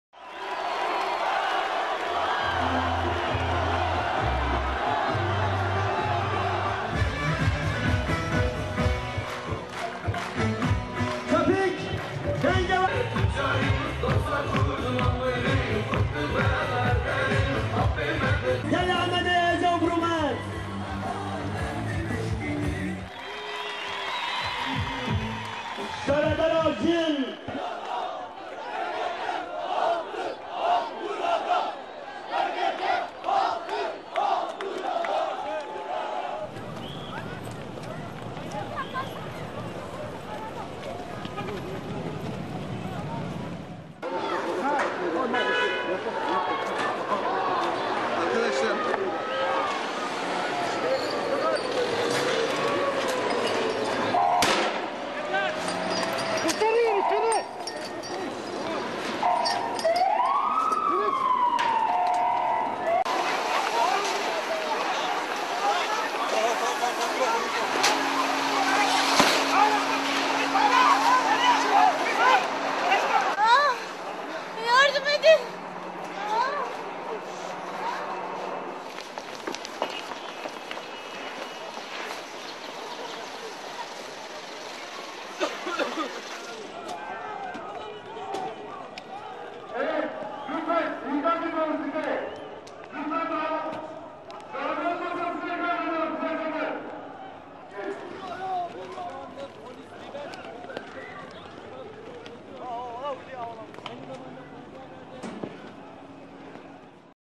Diyarbakir - Turkey
Kurdish protesters clash with police who used Water cannon, tear gas and batons to stop the protest.